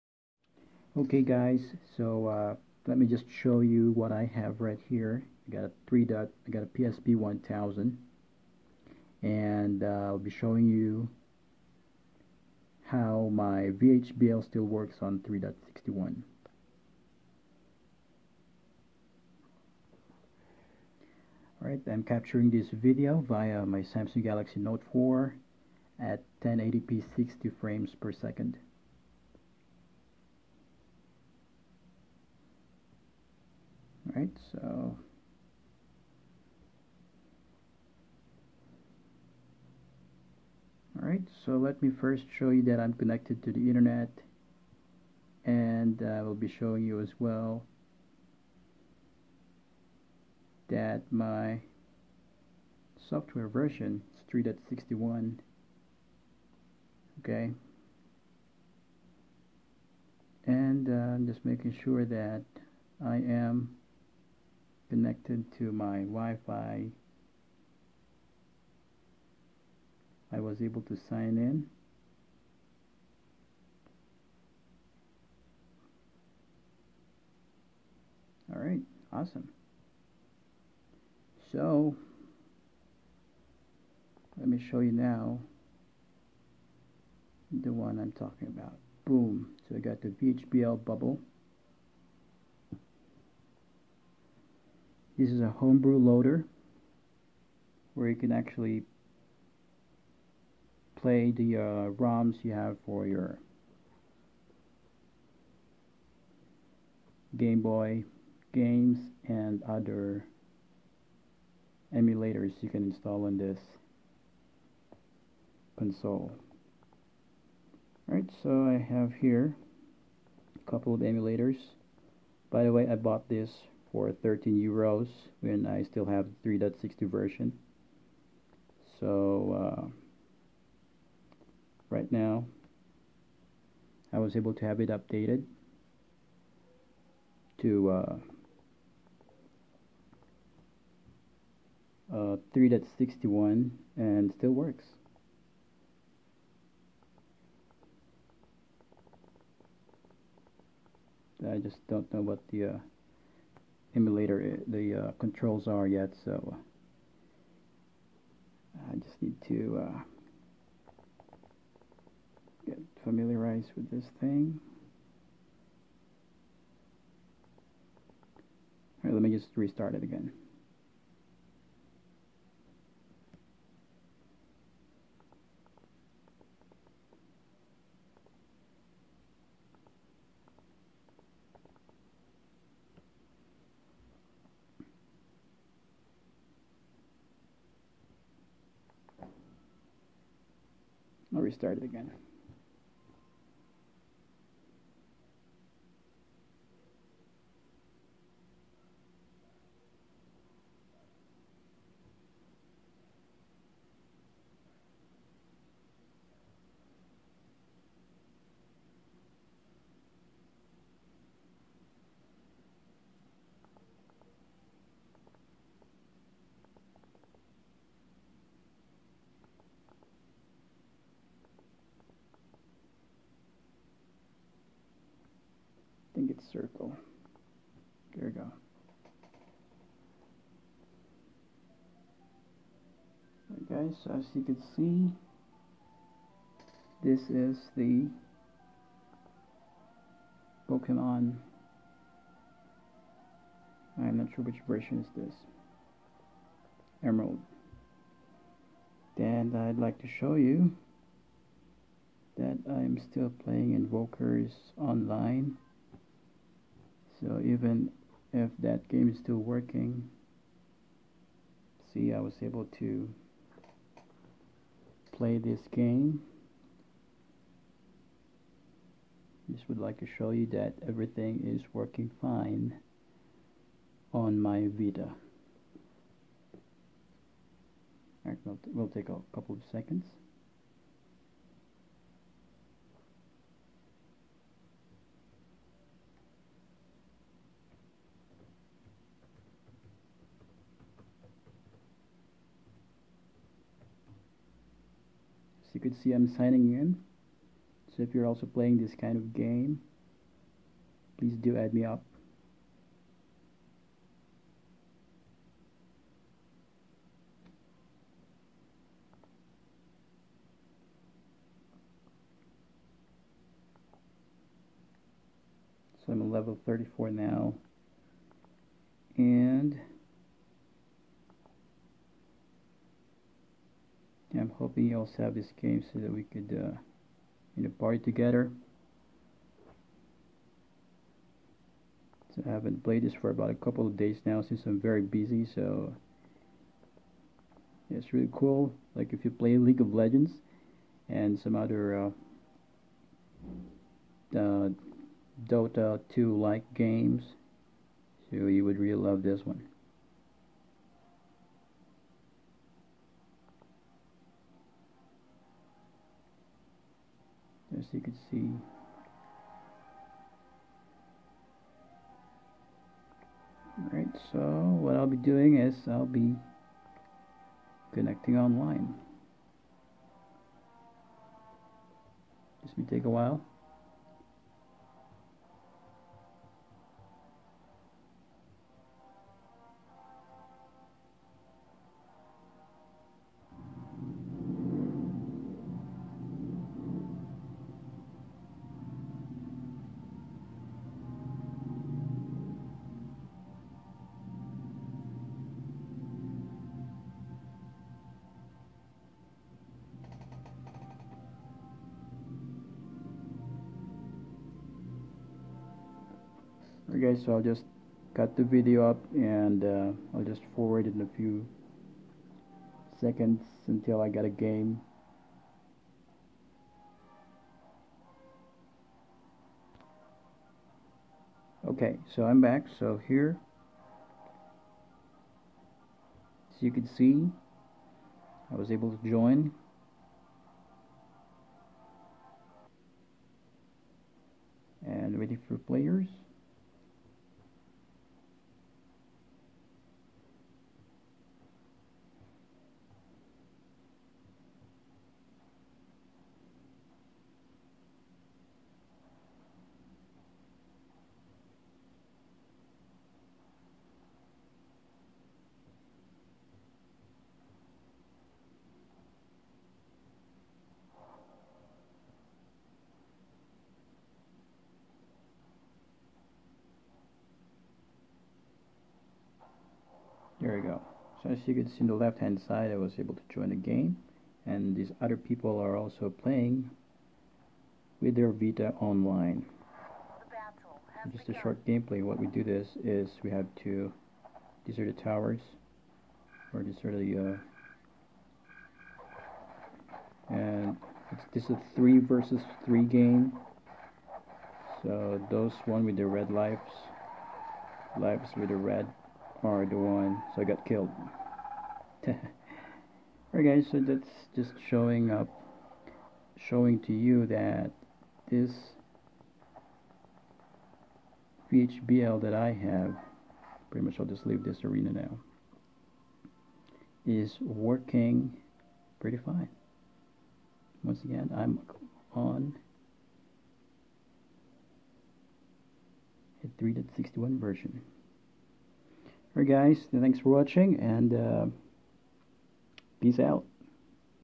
PSvita Homebrew on 3.61
Thanks to energymodding
My homebrew installation on 3.60 still works on 3.61

I have installed my VHBL before I upgraded to 3.61.
for those who dont who dont speak English I said my VHBL on my 3.60 STILL WORKS ON 3.61.

this is the website where is bought it from when henkaku is not available yet.